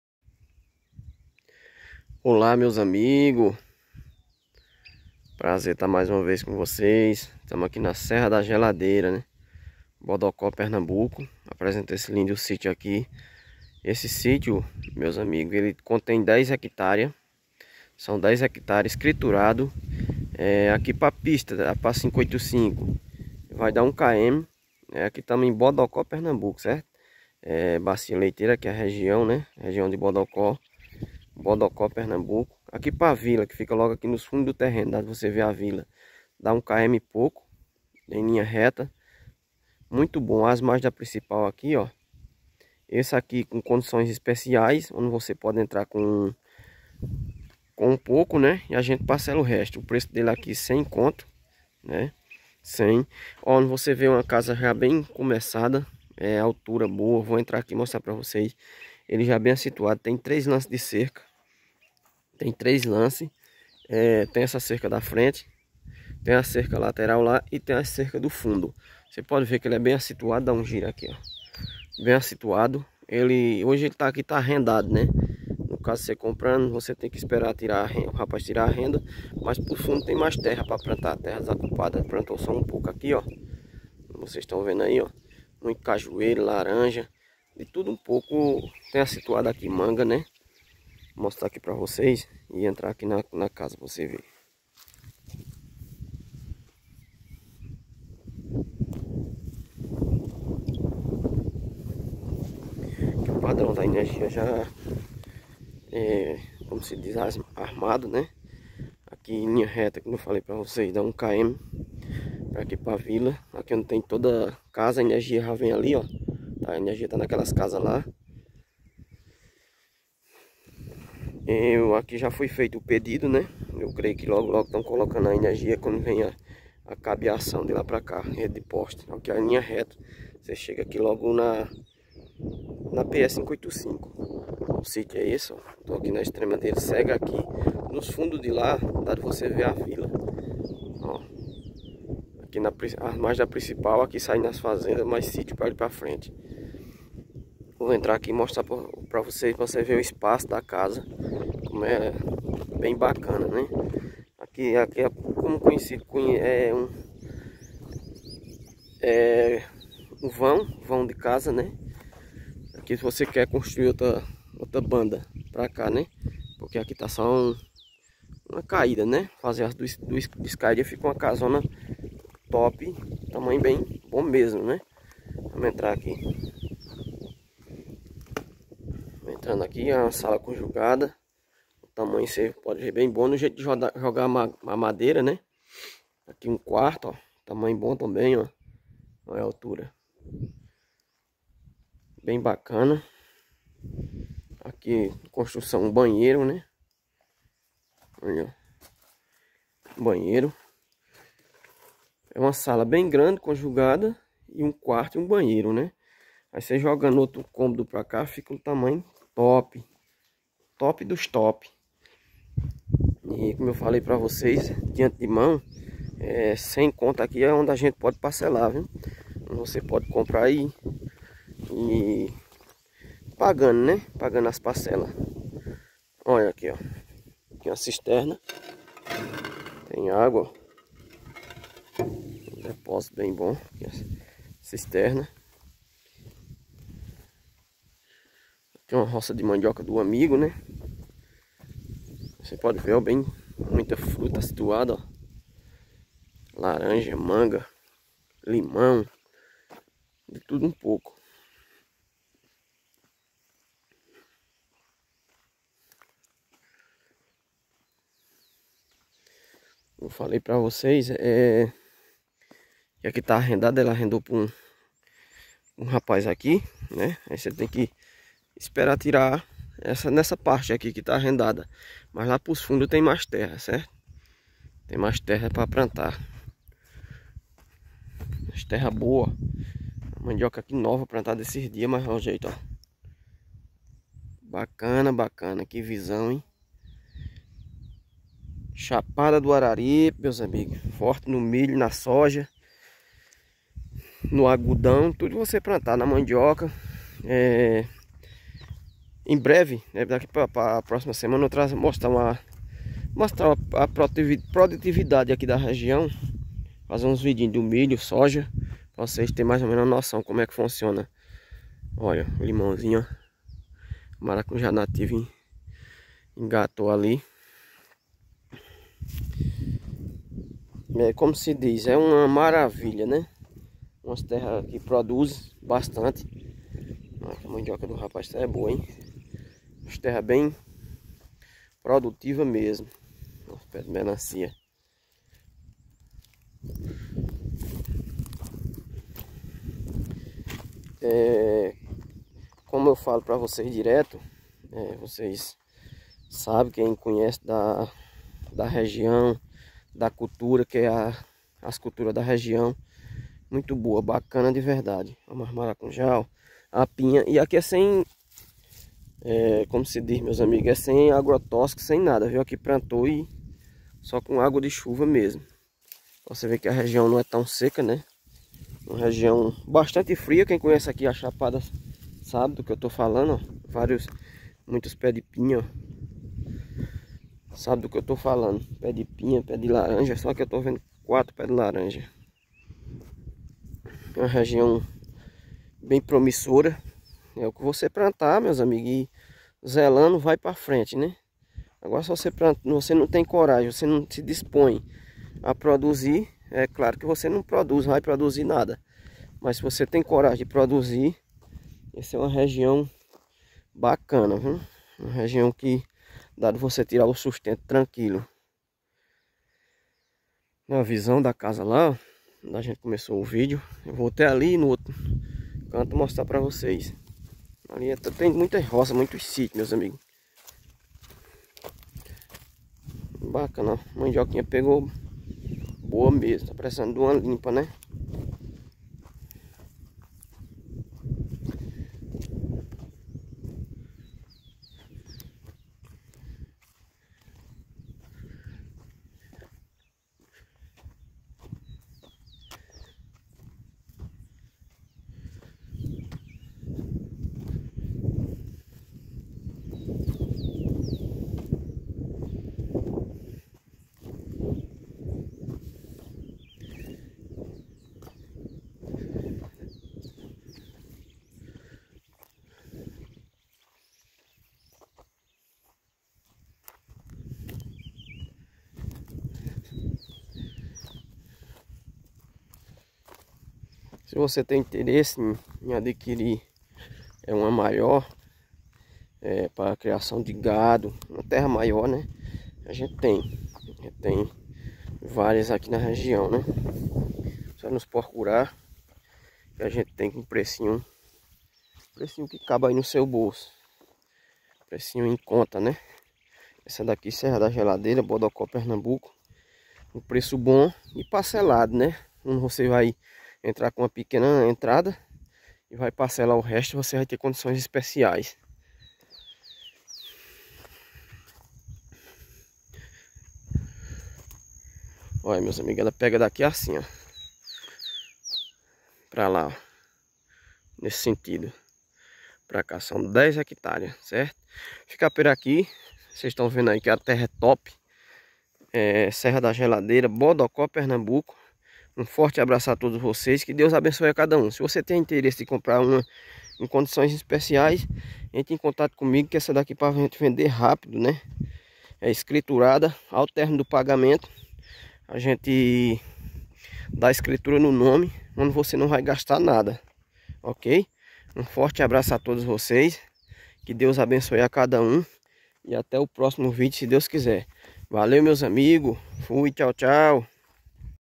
VENDO ESSE SÍTIO 10 HECTARES PARCELADO SUA GRANDE OPORTUNIDADE 8799102 3154
vendo esse parcelado tamanho 10 hectares
localizado na serra da geladeira bodoco pé escriturada
valor 100 mil
contatos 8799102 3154  / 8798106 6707  zap